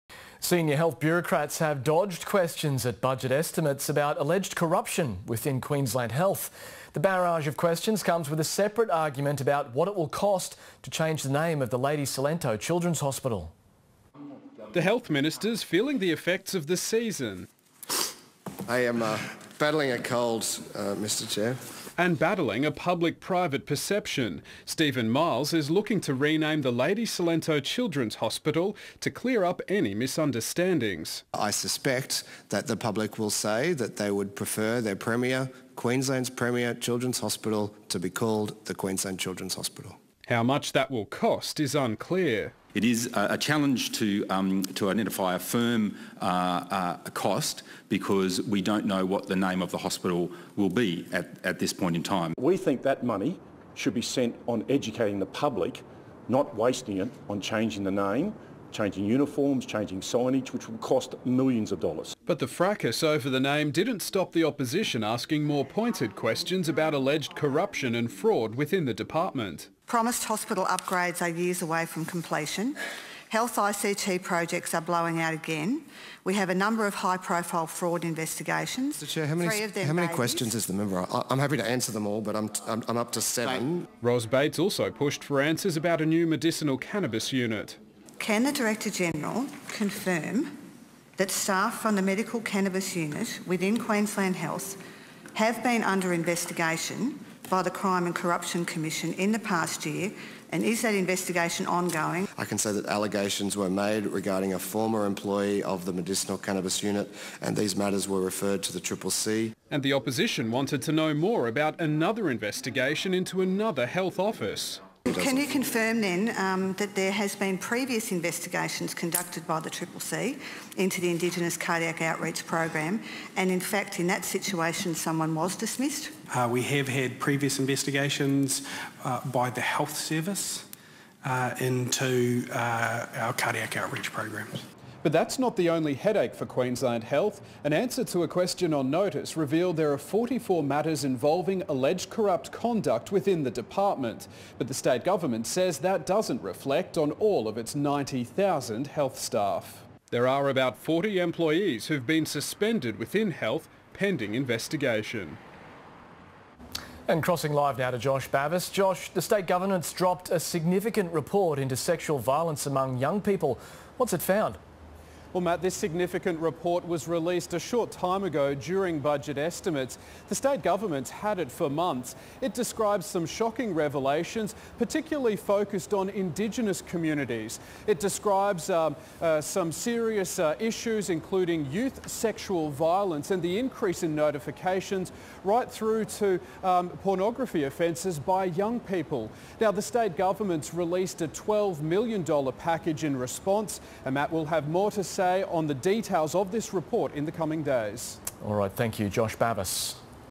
Petition from clinicians to rename LCCH to the Queensland Children's Hospital announced
31/07/2018 Estimates also hears that 44 matters are before the CCC over alleged corruption in Queensland Health.